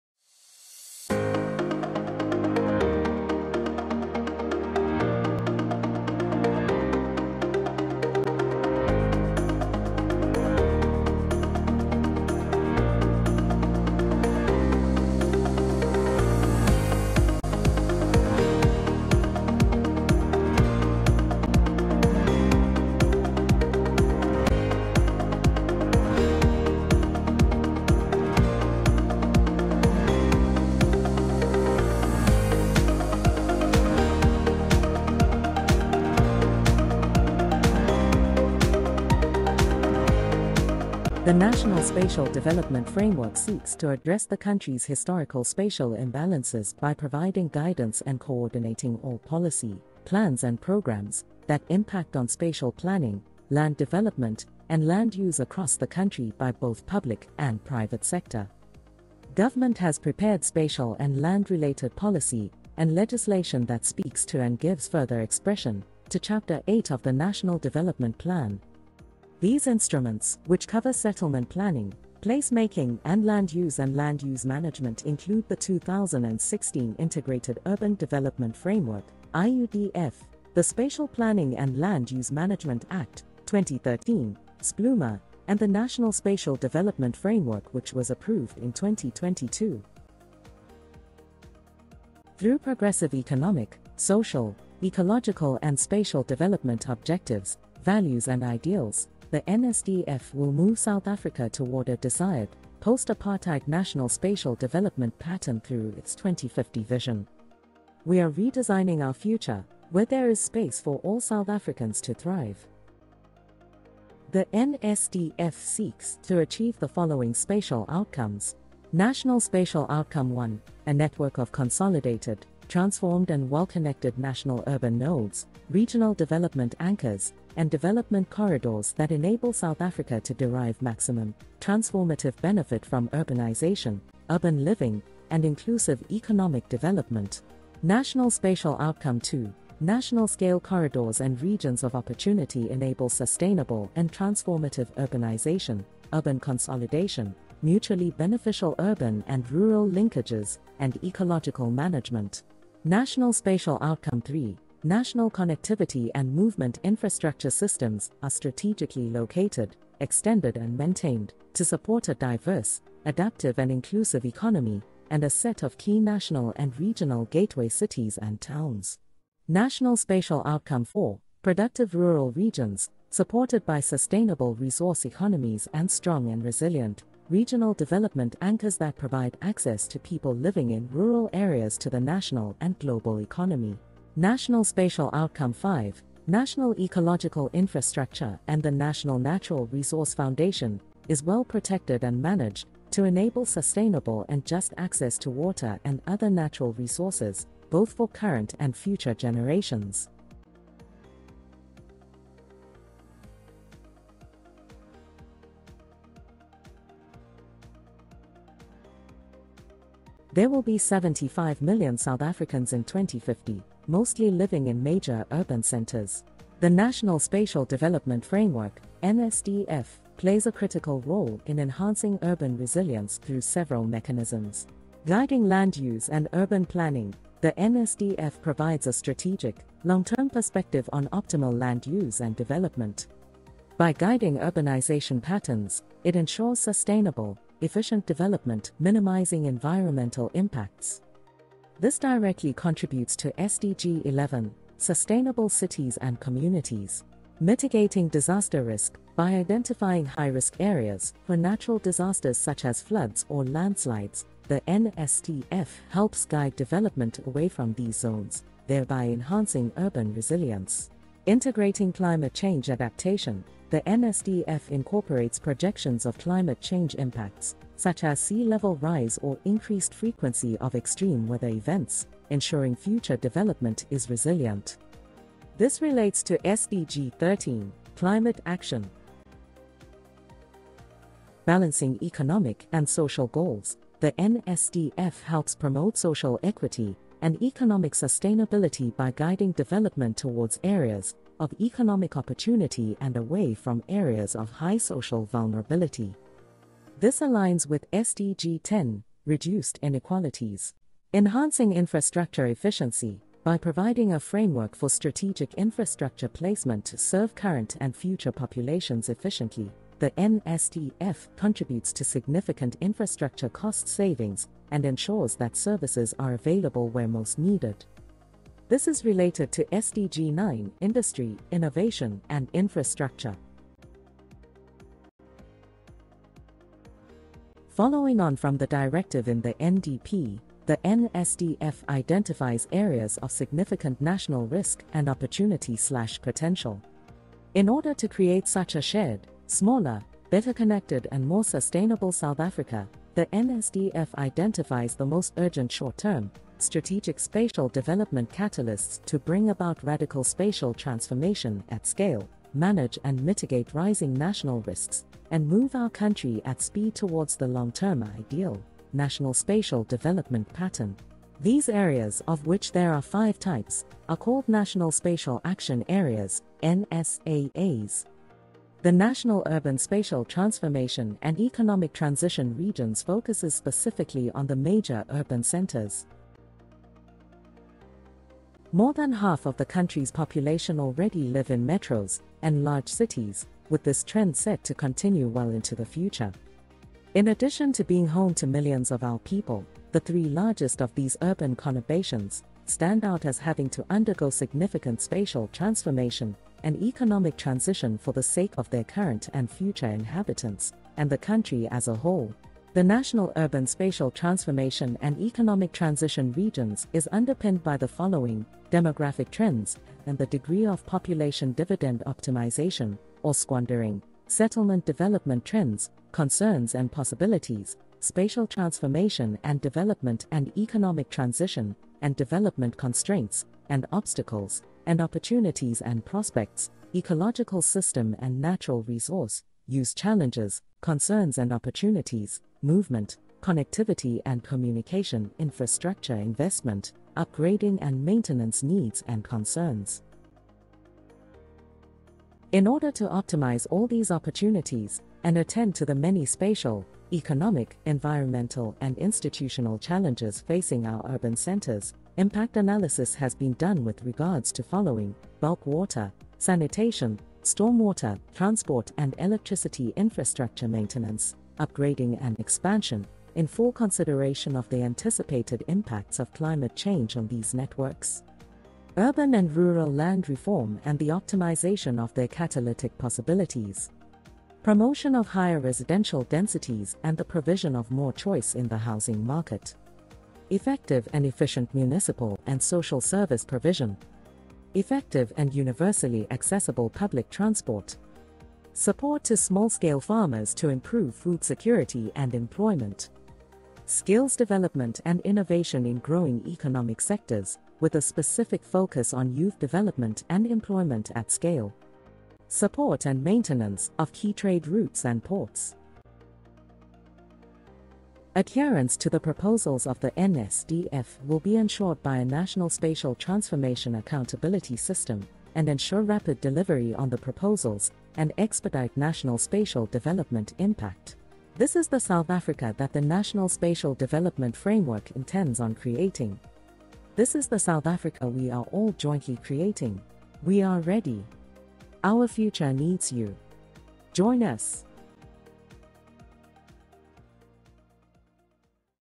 National Spatial Development Framework
The National Spatial Development Framework (NSDF) is the first of its kind to be compiled in terms of the Spatial Planning & Land Use Management Act (SPLUMA), a new addition to the ‘family’ of spatial development frameworks (SDFs) in the Republic of South Africa.